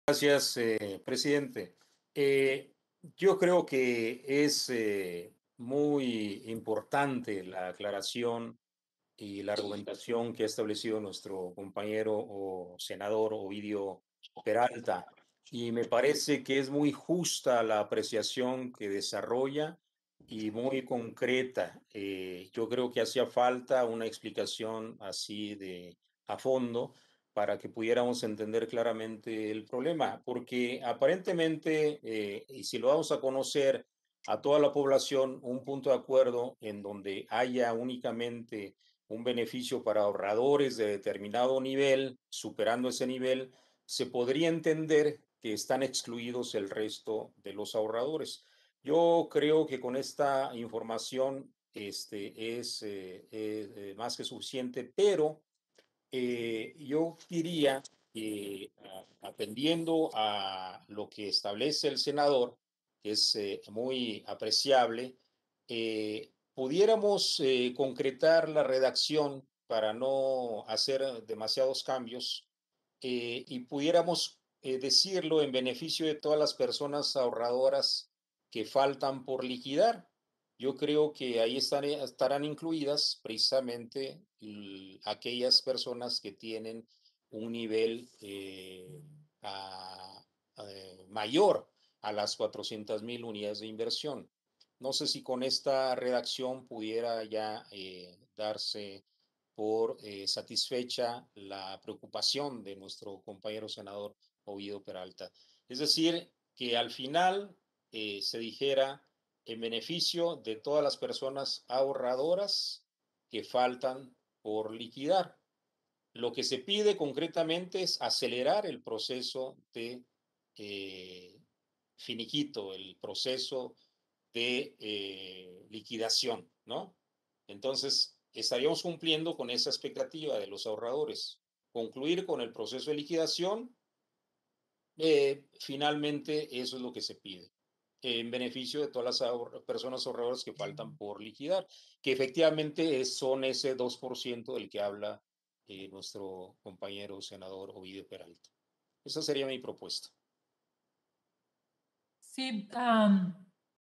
22/07/2021 Segunda participación del Dip. Marco Antonio Medina Pérez
Segunda participación del Dip. Marco Antonio Medina Pérez, durante la Reunión de Trabajo de la Tercera Comisión de la Comisión Permanente.